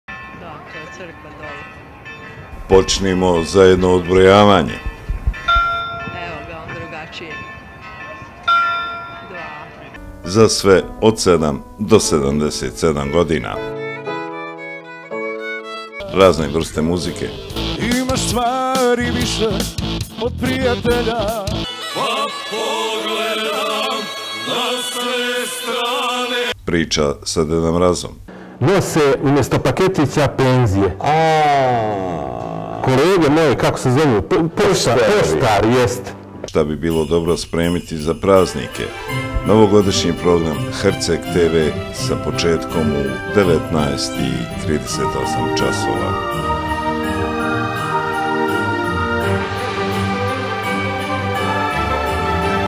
NAJAVA: Novogodišnji program na Herceg RTV
Novogodišnji program samo na Herceg televiziji , 19.38časova.
Herceg televizija i ove godine snimila je veoma interesantan novogodišnji program koji će se emitovati u najluđoj noći. Okupili  smo sjajnu ekipu, koja se pobrinula da gledaoci ostanu pored malih ekrana i uživaju u odličnoj pjesmi, plesu ali i zabavnim temama koje su prikladne baš za novogodišnju noć.
Sa željom da ugodno dočekate Novu godinu, i da nam ona bude mnogo bolja od ove 2022. za koju se mnogi raduju da je ispraćamo, pozivamo vas da budete uz novogodišnji program Herceg televizije.